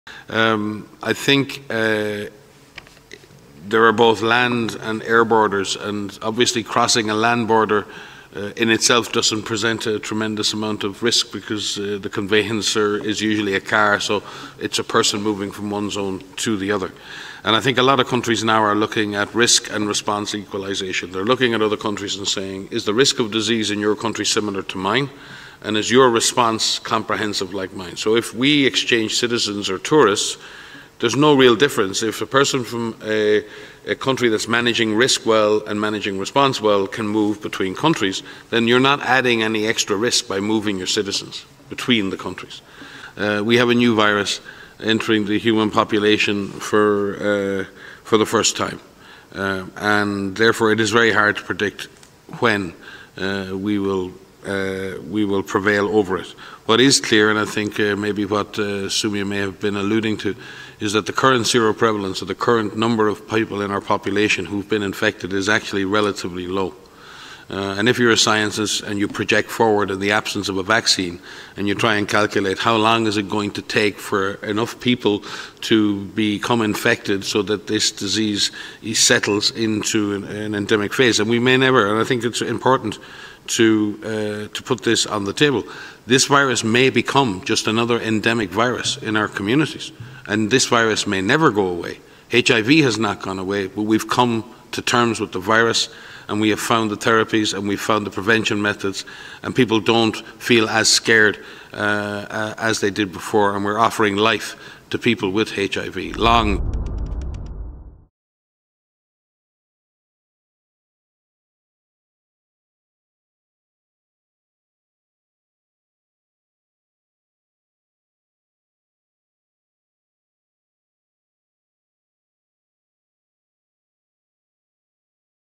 La OMS: El coronavirus podría convertirse en un virus endémico y no irse nunca
El nuevo coronavirus, causante de la enfermedad COVID-19, puede convertirse en un virus endémico y, por tanto, "no irse nunca", dijo hoy el director de Emergencias Sanitarias en la Organización Mundial de la Salud (OMS), Mike Ryan.